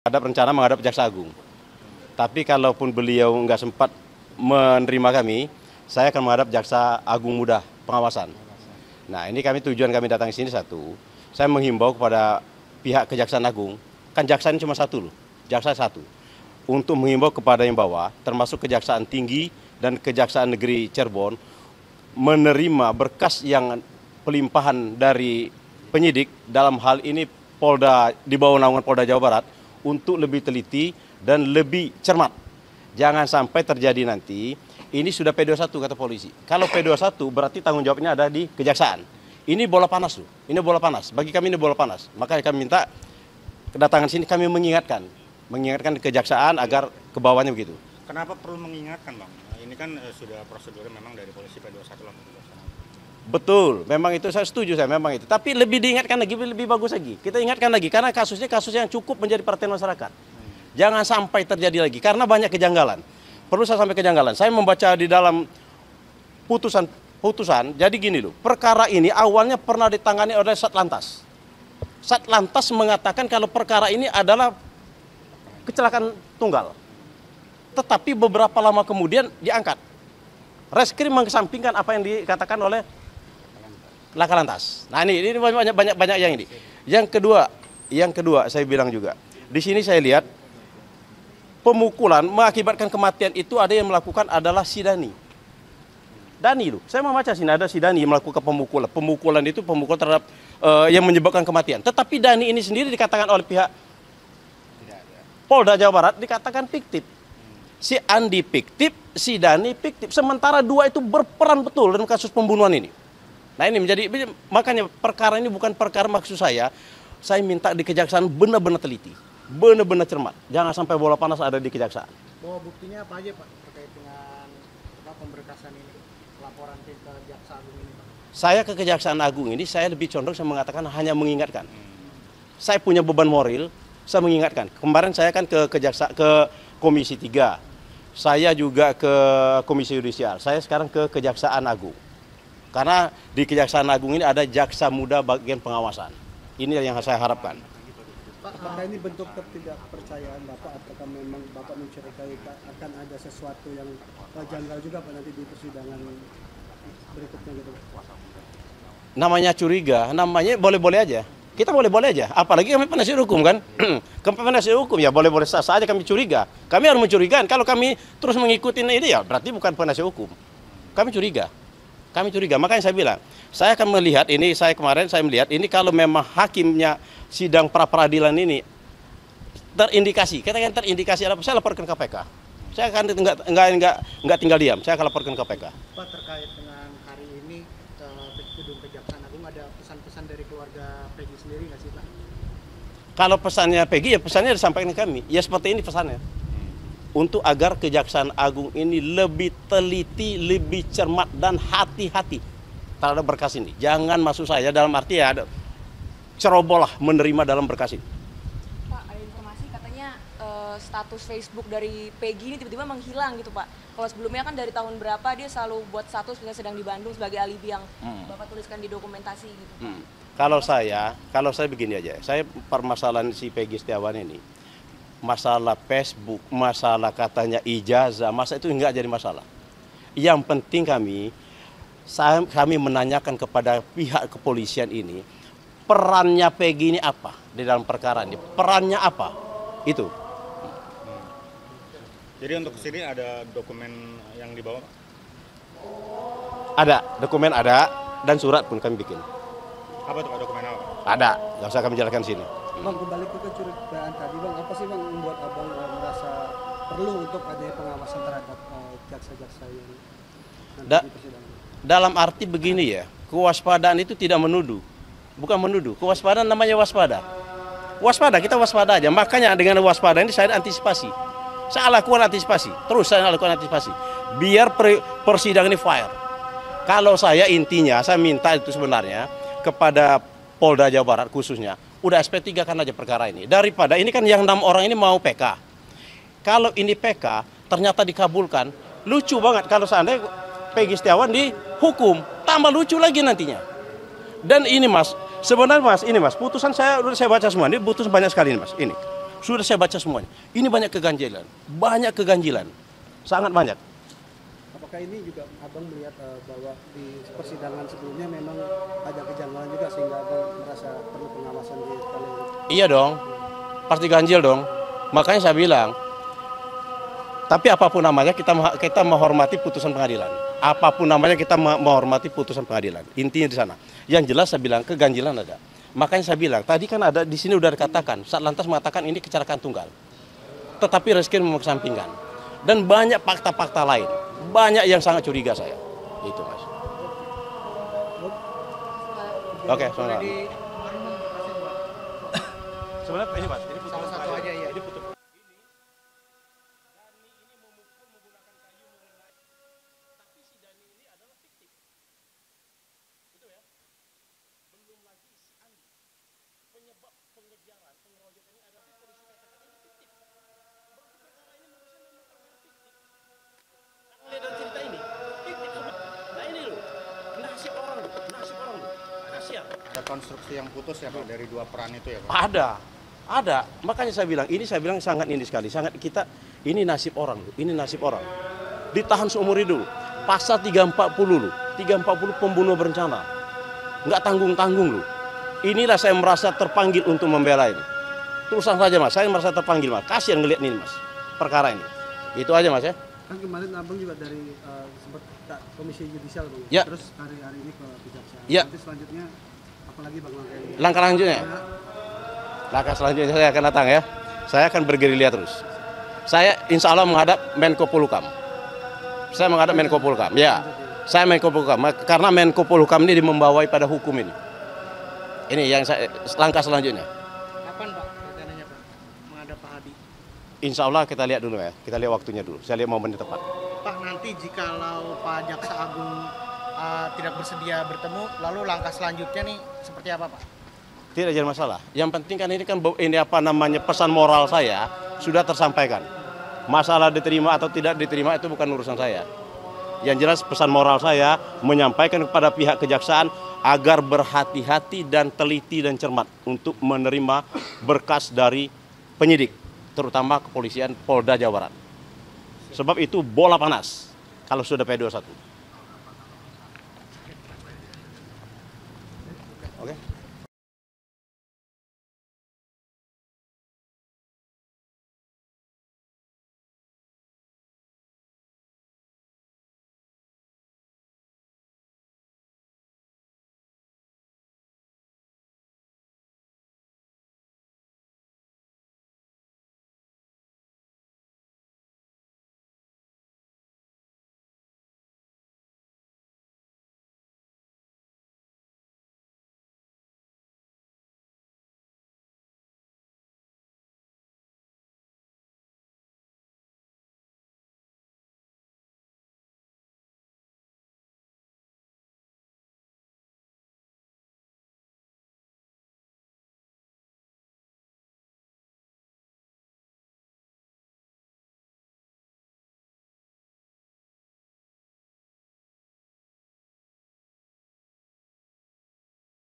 Pengacara Pegi Ingatkan Jaksa Cermat Tangani Kasus Kliennya: Jangan sampai Terulang seperti 2016
Tim Kuasa Hukum Pegi Setiawan, tersangka kasus Vina Dewi alias Vina Cirebon, Mayor TNI (Purn) Marwan Iswandi menyambangi Kantor Kejaksaan Agung (Kejagung), Jakarta, Rabu (19/6/2024).

Marwan mengatakan pihaknya sengaja datang ke Kejagung untuk mengharapkan perhatian Jaksa Agung Santiar soal kasus kliennya. Ia berharap agar Kejaksaan Negeri Cirebon khususnya dapat bekerja dengan cermat usai menerima pelimpahan berkas kasus Pegi dari Kejaksaan Tinggi Jawa Barat.

"Untuk mengimbau kepada yang bawah, termasuk Kejaksaan Tinggi (Jawa Barat) dan Kejaksaan Negeri Cirebon menerima berkas yang pelimpahan dari penyidik dalam hal ini di bawah naungan Polda Jawa Barat untuk lebih teliti dan lebih cermat," kata Marwan di Kantoredung Kejagung RI, Jakarta, Rabu (19/6/2024).